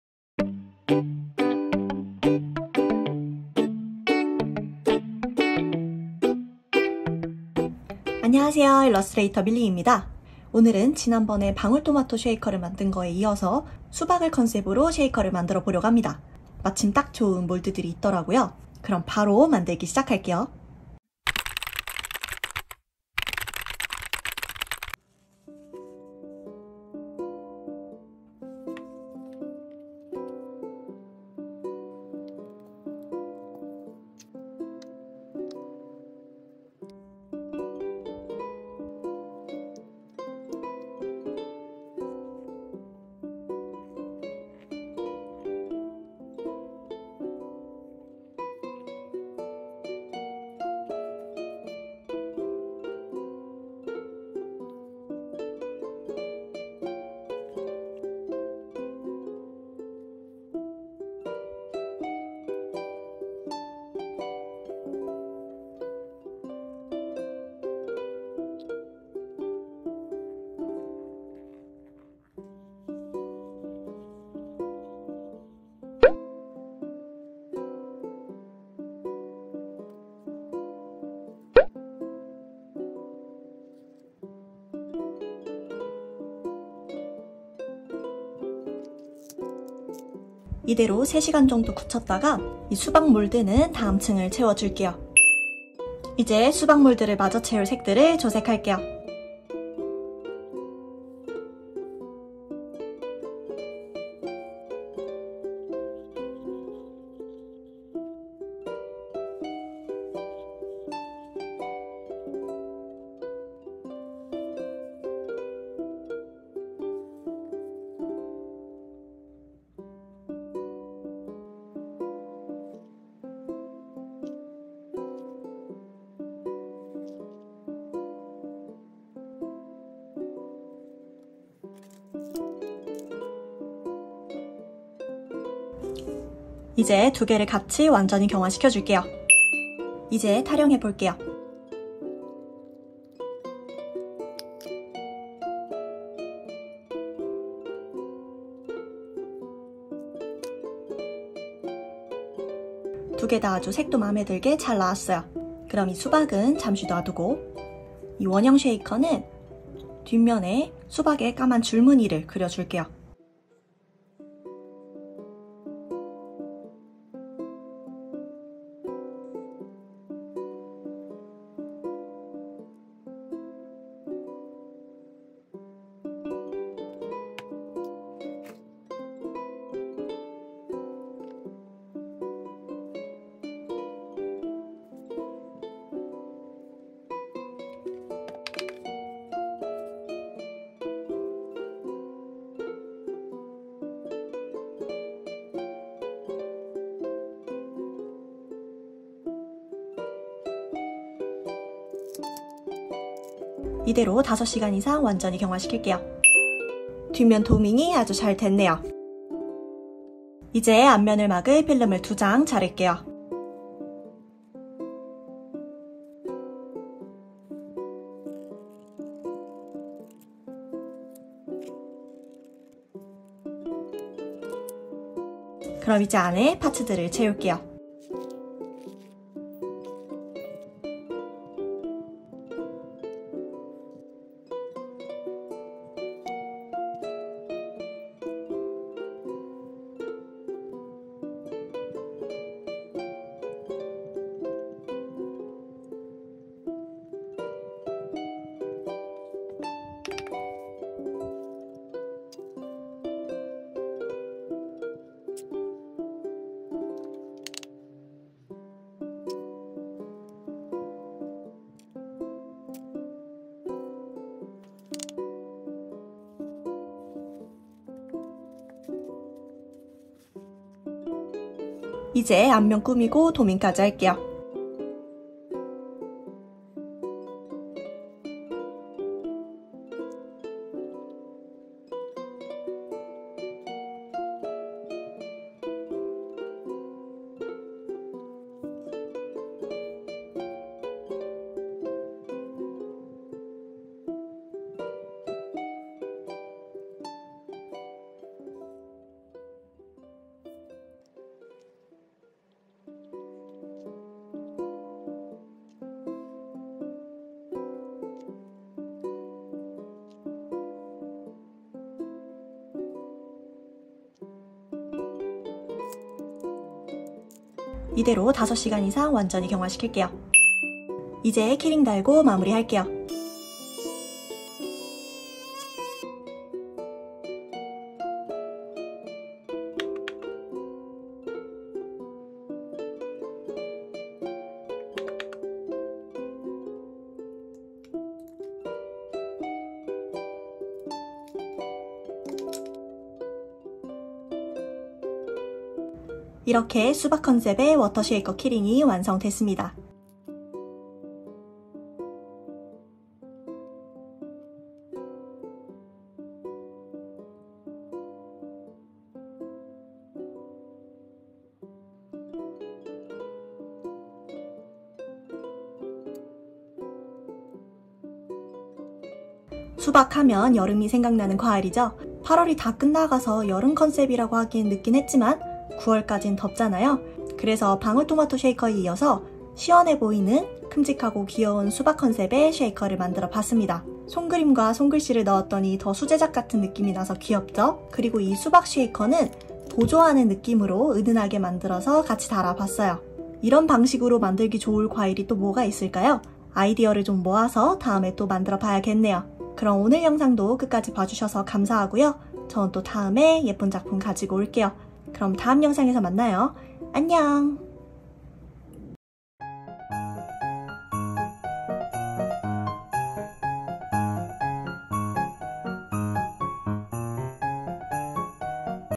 ENG l 레진아트 l 아직 너무 더우니까 🔥 보기만해도 시원한 수박 쉐이커 만들기 ! 🍉
▪️링크 모음

✨레진아트 재료구매처✨

파치타 레진
- 파치스토어 Phatsi Store (Phatsita resin)

아크릴 마카 & 슬림 원형 쉐이커 몰드
- 마니랜드

수박 폴리머 파츠 & 반짝이 스티커
- 슈가데코

수박 몰드
- 옥상공방스토리 (큐티 쉐이커 몰드2_수박)

✨Where to buy resin art materials ✨

Phatsita resin
- Phatsita Store

Acrylic Marker & Slim Round Shaker Mold
- Maniland

Watermelon Polymer Parts & Glitter Stickers
- Sugar Deco

Watermelon mold
- OSGB Story (Cutie Shaker Mold 2_Watermelon)